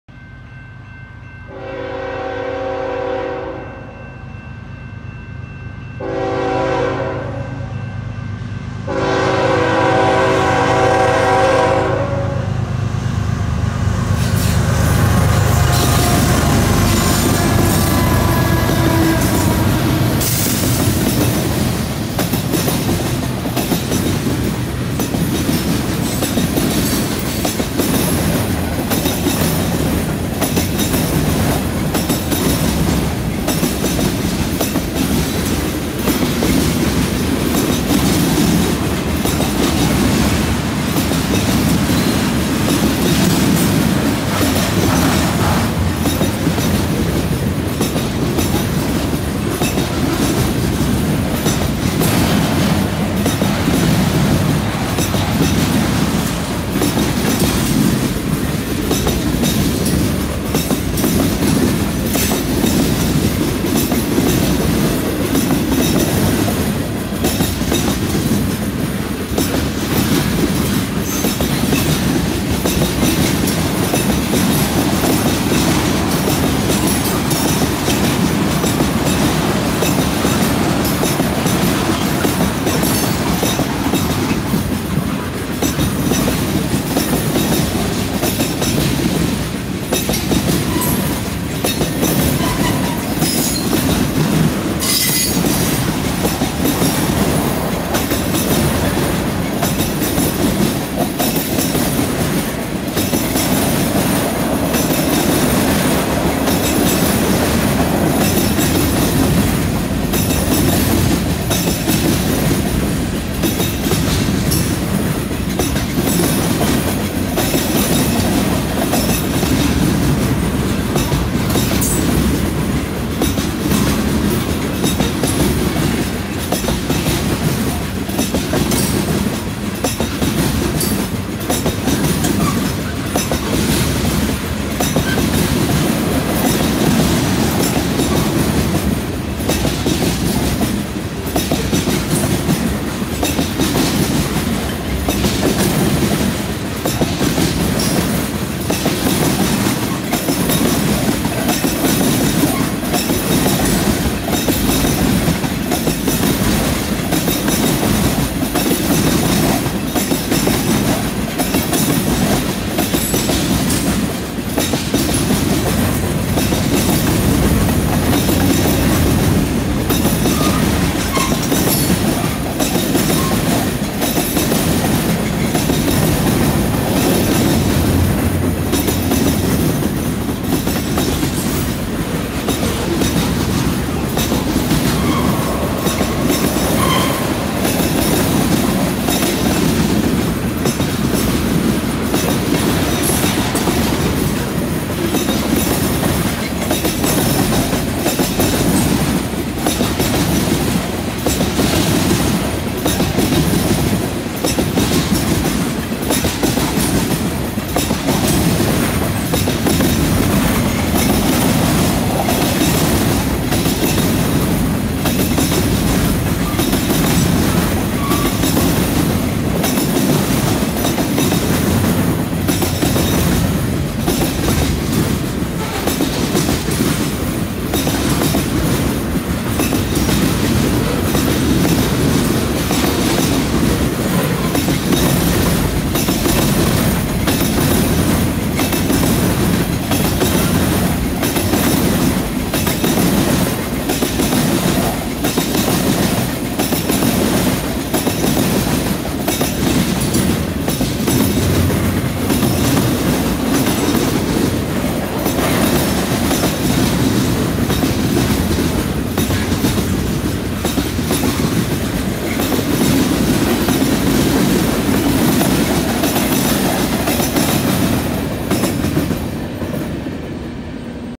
3/16/22 CN Train 397
3/16/22 16:11 64 balmy degrees at CN Charlotte as CN 2991, CN 5661, and CN 5652 power Train 397 over the ChS junction switch and past the old GTW depot. VC tankers on this train. Full 4:31 video: